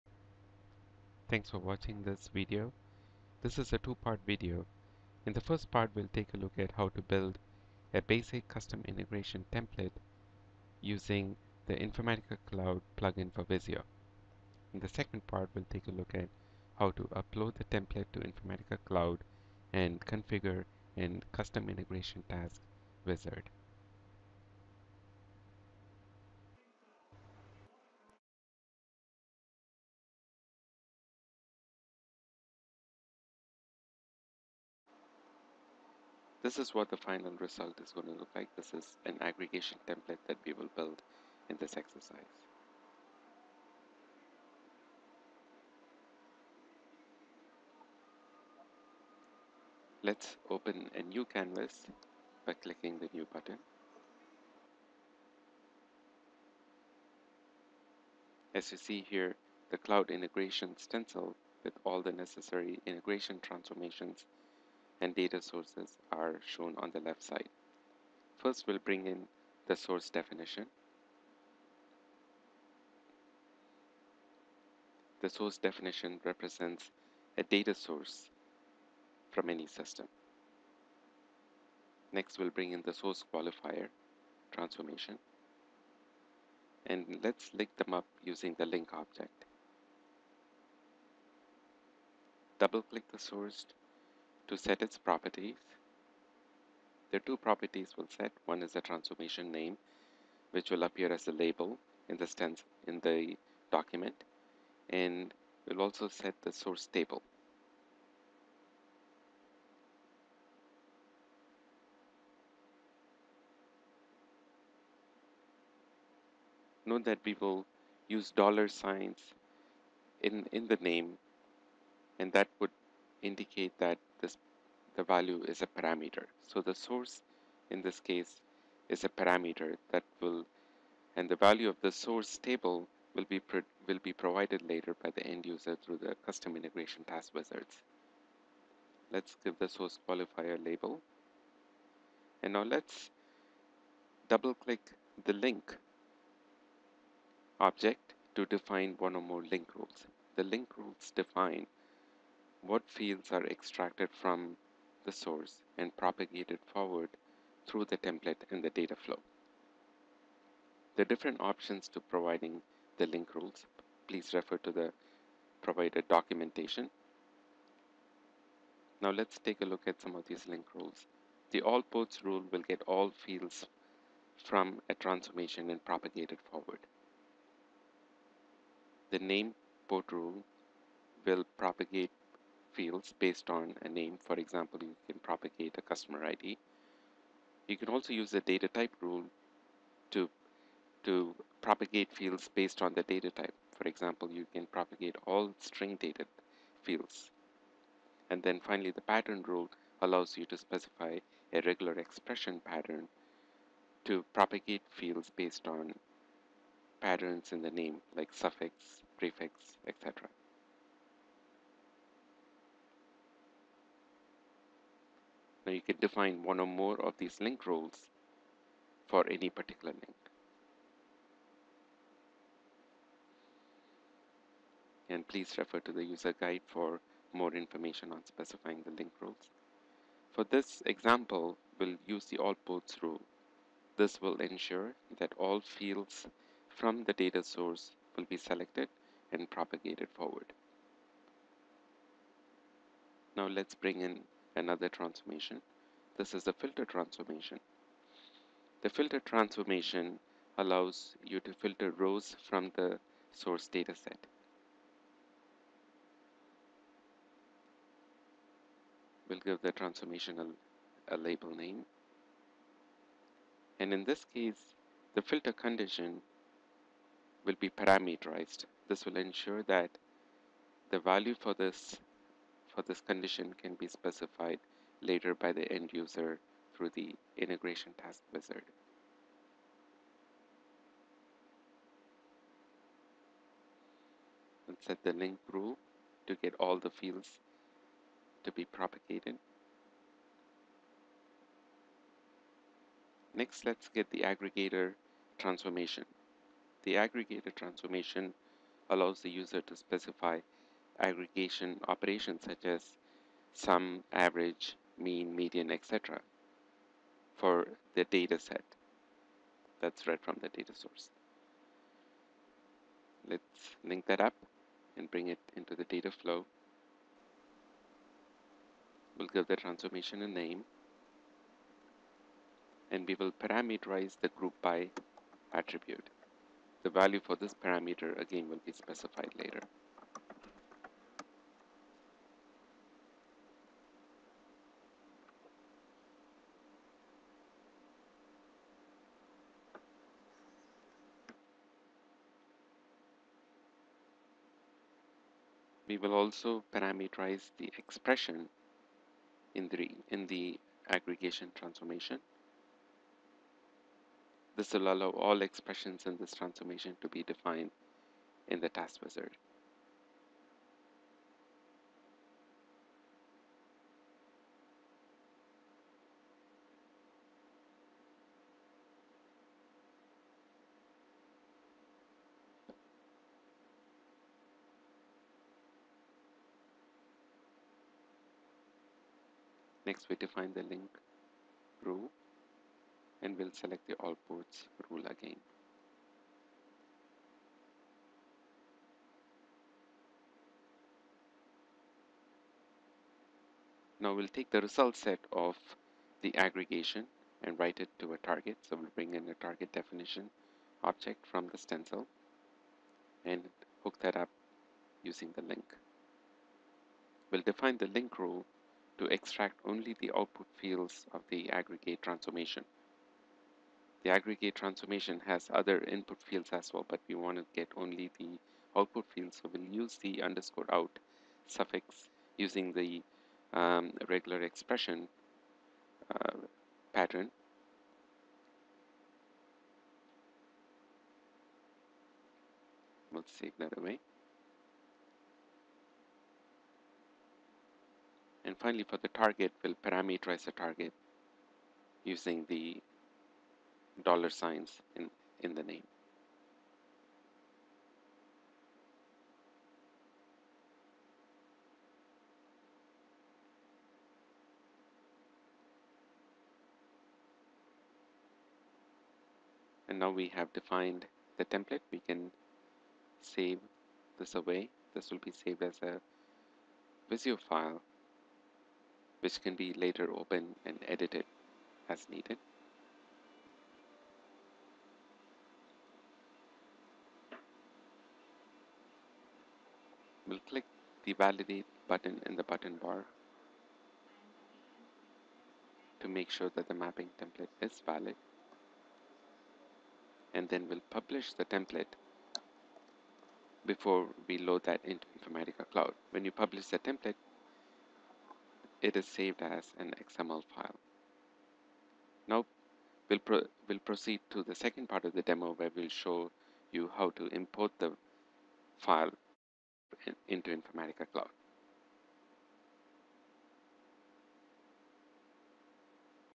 Creating Templates with Informatica Cloud's Template Designer
There are a variety of Cloud Integration Templates available on the Informatica Cloud Marketplace but what if you want to build your own integration template from scratch? Here's a step by step guide on how to do it yourself in under 10 minutes. To download the Informatica Cloud Template Designer, go